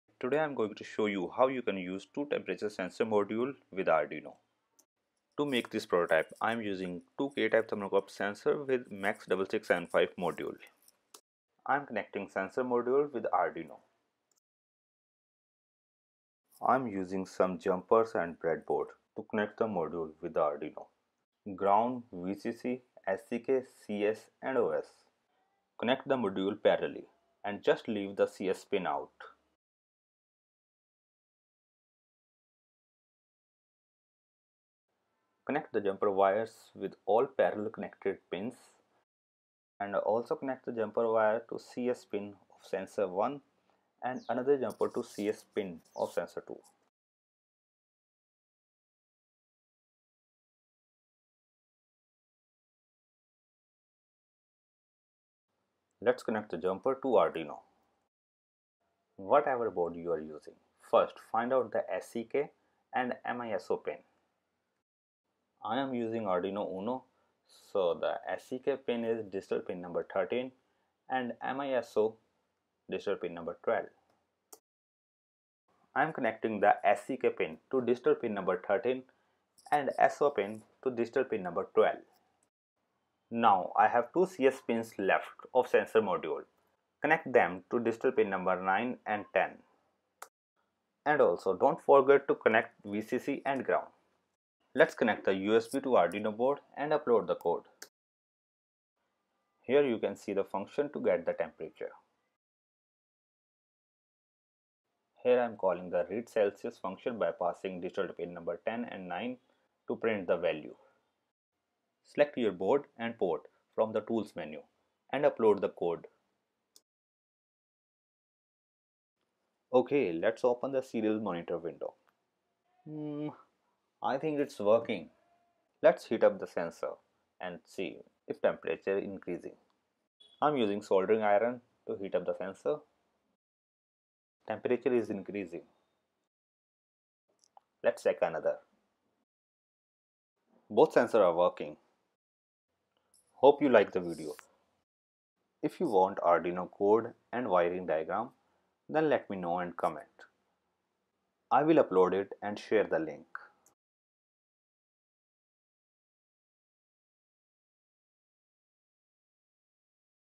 Two Temperature Sensor Module MAX6675 with Arduino
In this video you can see how I am using two K-Type temperature sensor Module MAX6675 with Arduino.